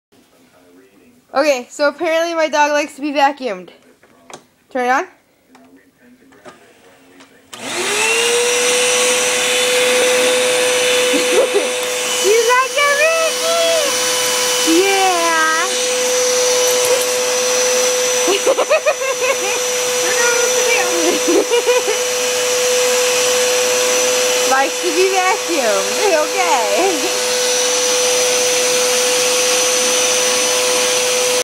Ricky likes the vacuum
My dog Ricky likes to be vacuumed